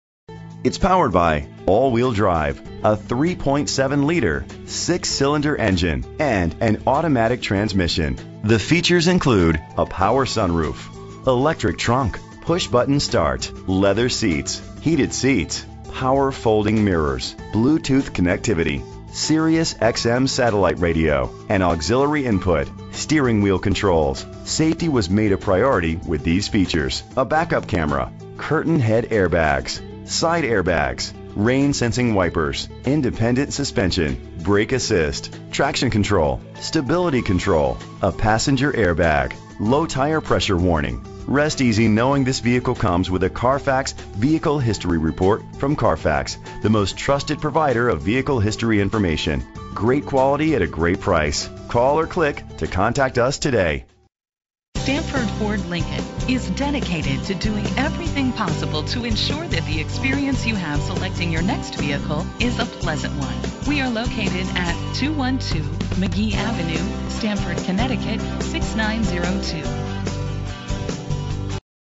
2012 Infiniti M37x - Stamford CT
Phone:
Year: 2012
Make: Infiniti
Model: M37x
Trim:
Engine: 3.7 liter 6 cylinder 24 valve
Transmission: 7-Speed Automatic
Color: Harbour Blue
Mileage: 69311
Address:
212 Magee Avenue